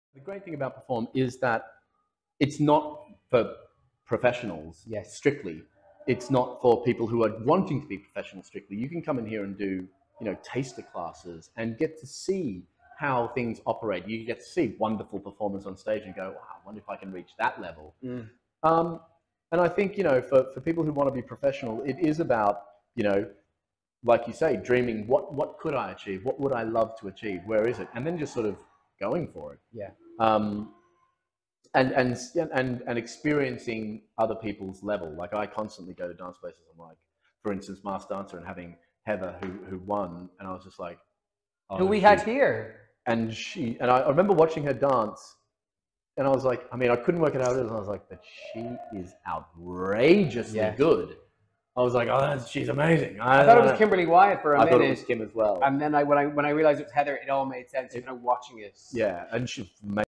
Throwback to Adam Garcia talking to Paul Ryder at Perform 2022!
Get ready for a star-studded experience at Perform Scotland 2023 because Adam Garcia is coming!! 🔥 ⁠
But first, take a look at this sneak peek of Adam chatting with Paul Ryder at Perform Festival, Dublin in November 2022, where he spent a busy day teaching dance and acting workshops.
With a list of credits that include Coyote Ugly, Wicked Musical, Saturday Night Fever, Masked Dancer, and many more, you won't want to miss this learning from this multi-talented sensation. ⁠Grab your tickets for Perform Scotland 2023 now before it's too late! 🤩🤩⁠